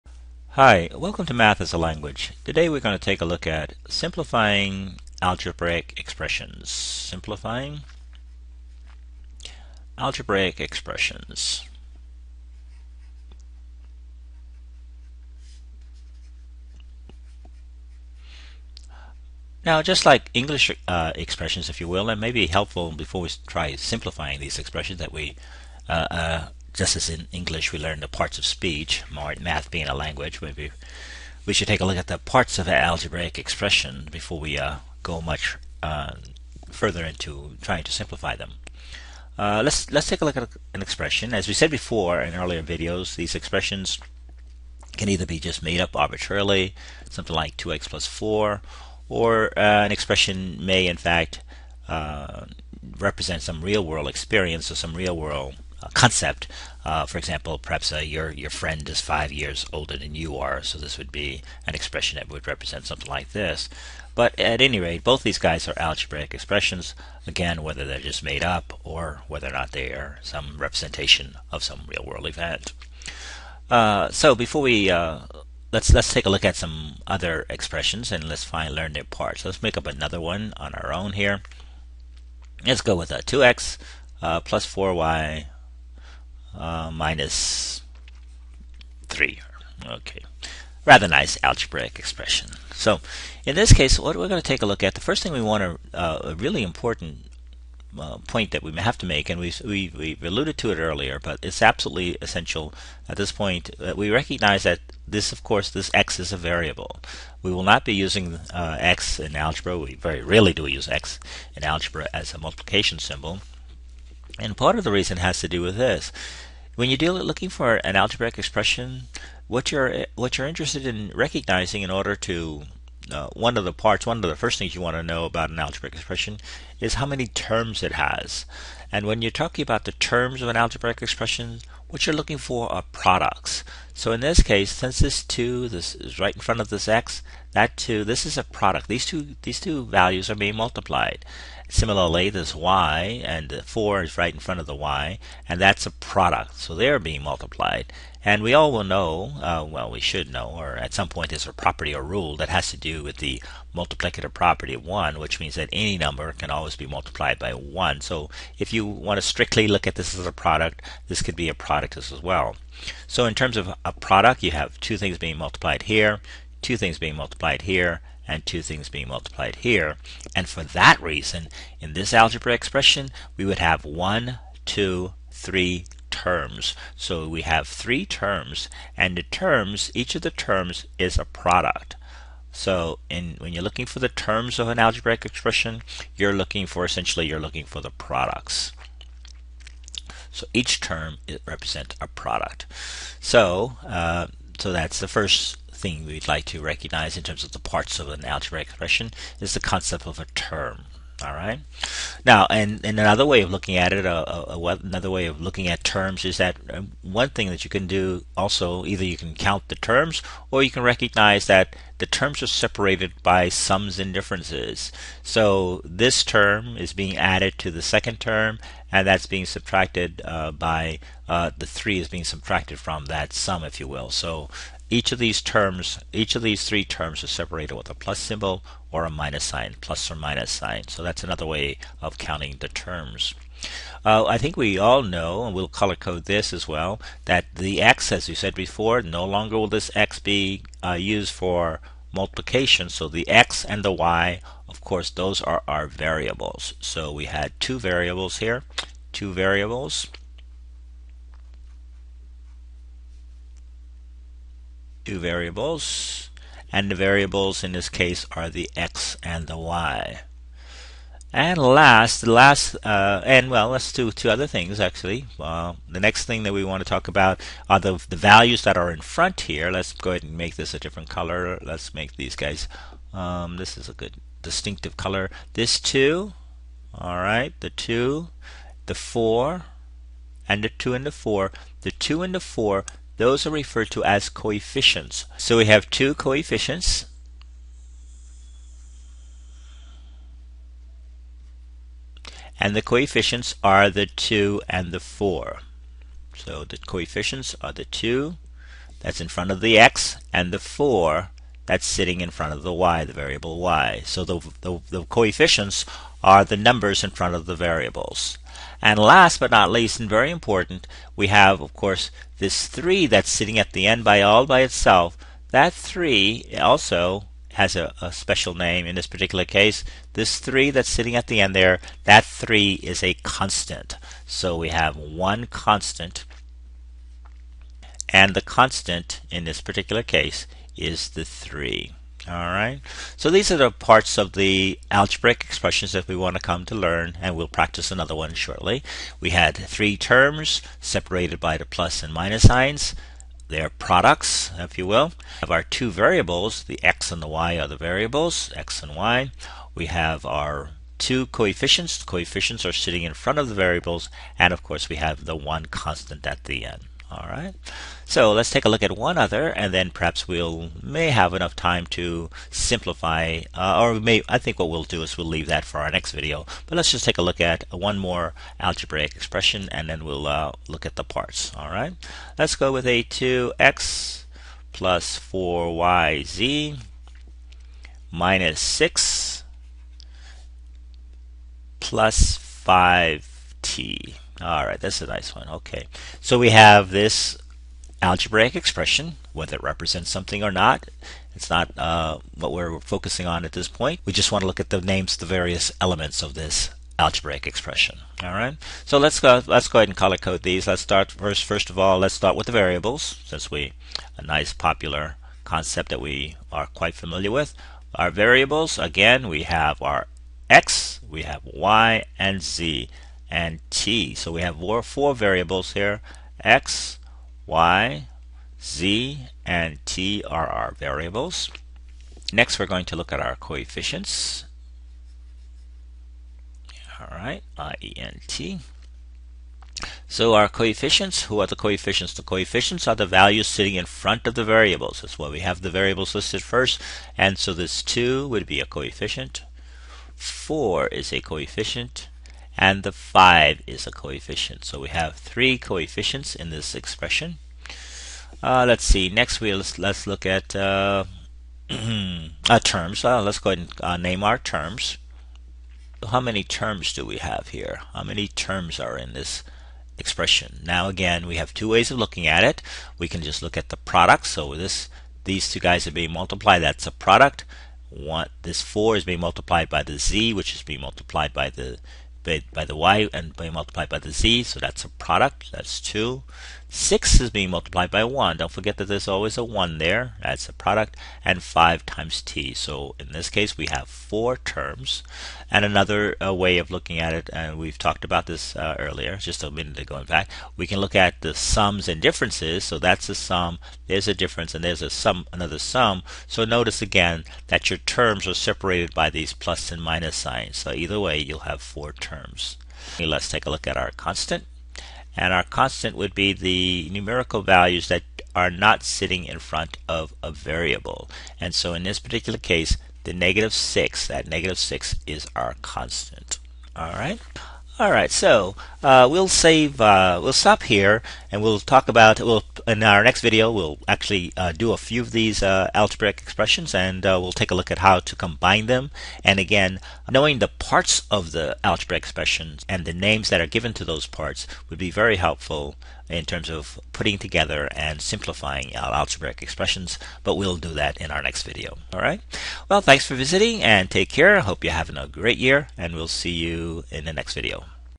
Identifying Parts of an Algebraic Expression
Knowing the parts of an algebraic expression is a good first step to simplifying them.